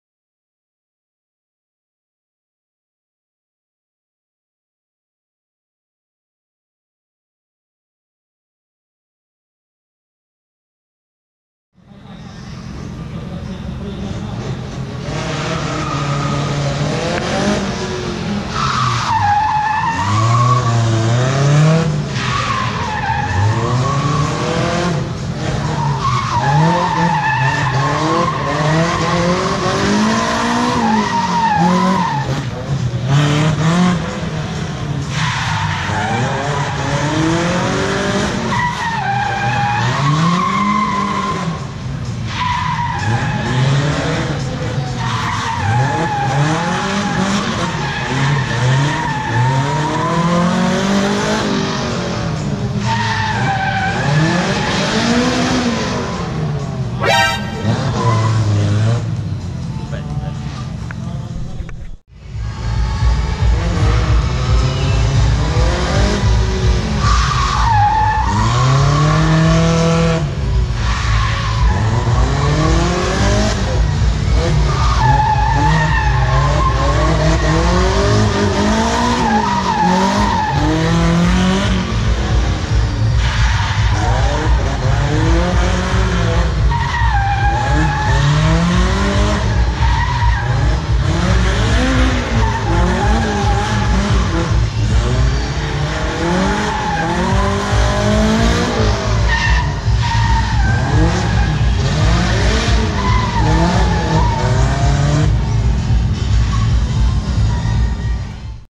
GYMKHANA/KONE Attack Championship 2017 Final Round☆Daihatsu Charmant
Date: 16 SEPTEMBER 2017
Venue: Padungan Community Council car park Kuching Sarawak
Event: KONE Attack Championship 2017 Final Round
Car: Daihatsu Charmant 4AGE (M)

Information:
WC - WRONG COURSE
DNF - DID NOT FINISH
# - 5 Seconds Finish Line Penalty
* - 2 Seconds Cone Penalty
** - 4 Seconds Cone Penalty
*** - 6 Seconds Cone Penalty
Cone falls after get hit = Penalty 2 sec
Cone moves outside the box completely after getting hit = Penalty 2 sec
Cone moves after getting hit but still staying within the box = No penalty
Front wheels must stop completely after the timer
Stopping the whole car after the timer = Penalty 5 sec